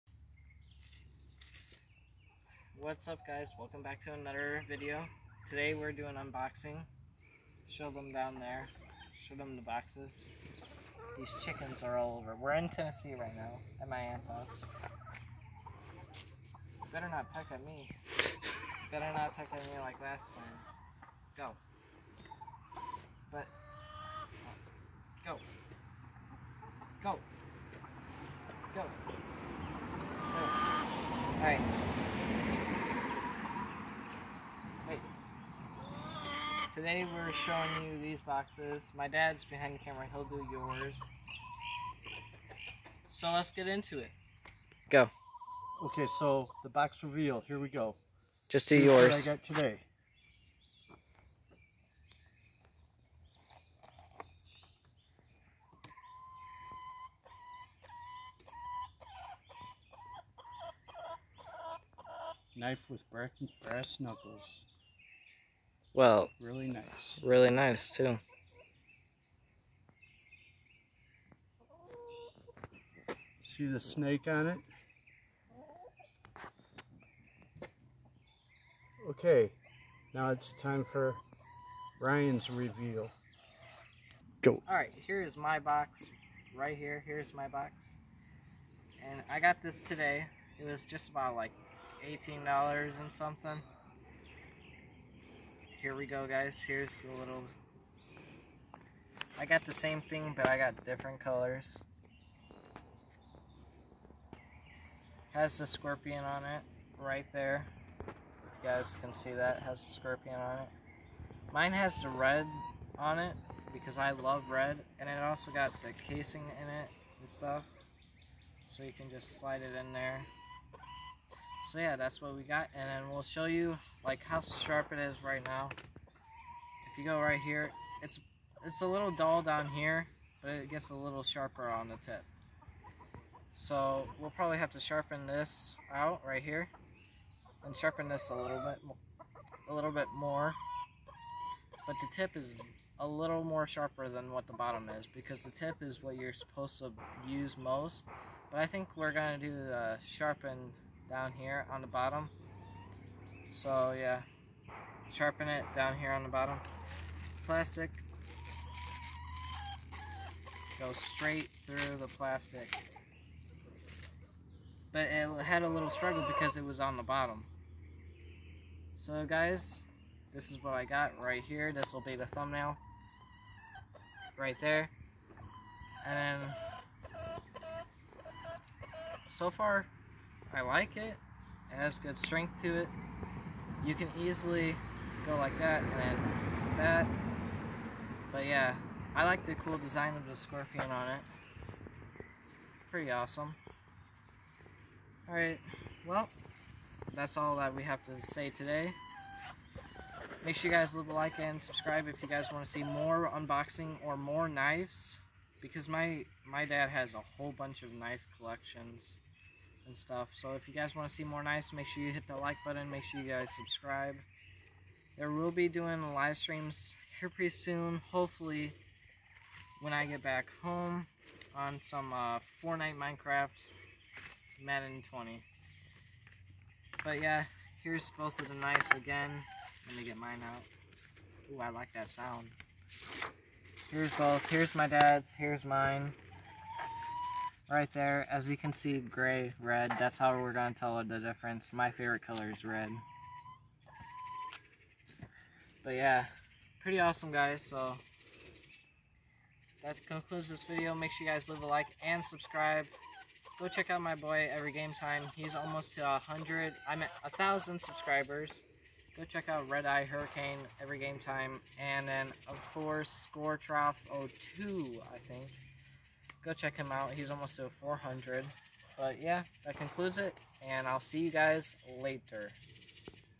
Box Openings!!!!
Me and my dad have two boxes to show u guys